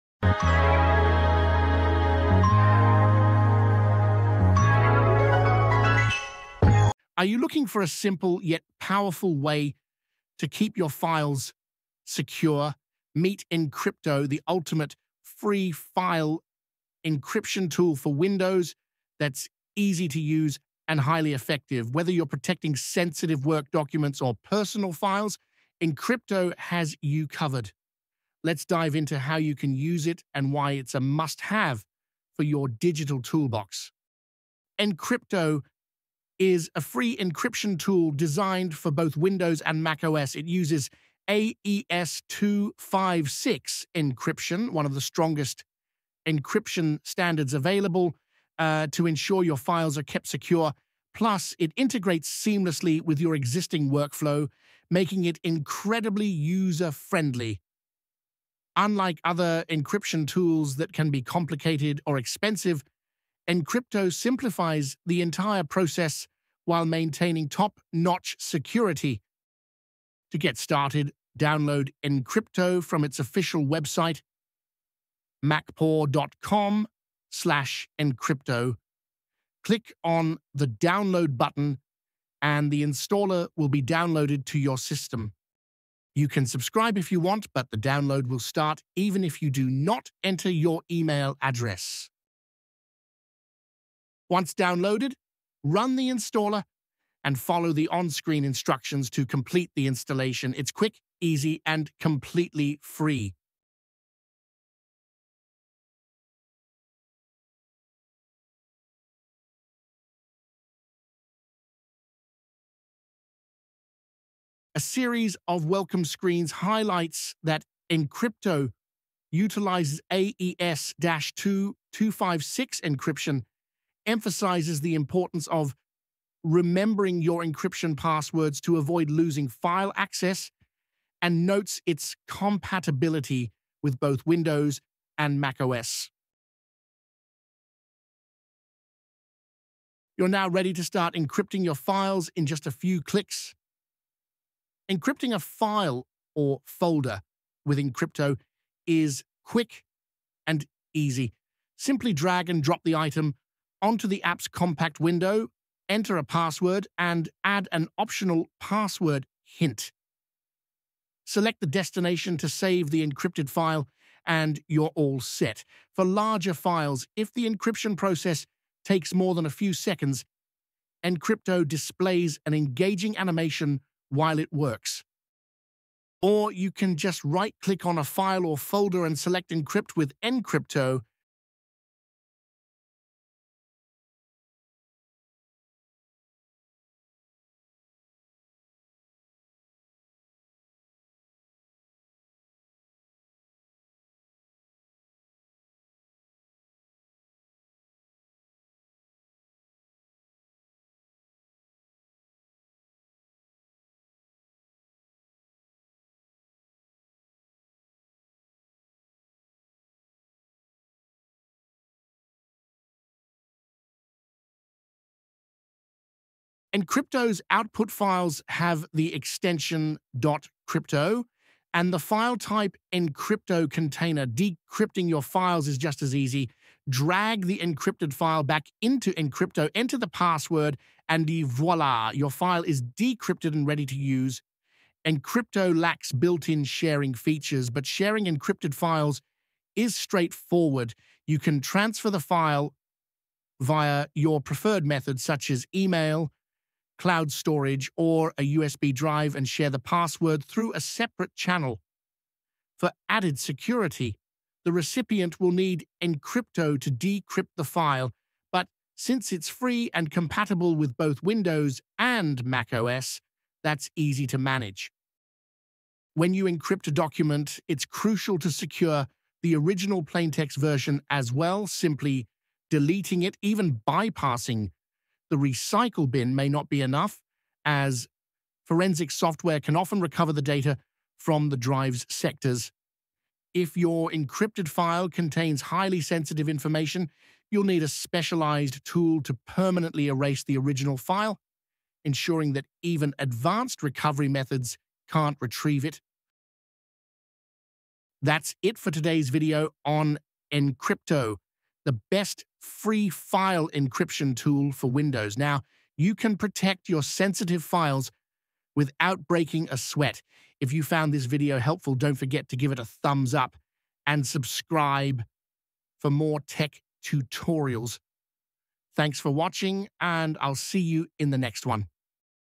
The BEST Free File Encryption Tool for Windows – Meet Encrypto
Looking for a reliable, user-friendly, and completely free way to protect your files? Look no further! In this video, we dive into Encrypto, the ultimate file encryption tool for Windows (and macOS) that combines simplicity with powerful security.

How Encrypto works to safeguard your sensitive files.
A step-by-step tutorial on encrypting and decrypting files.
Key features like password protection and file sharing.
Why Encrypto is perfect for both personal and professional use.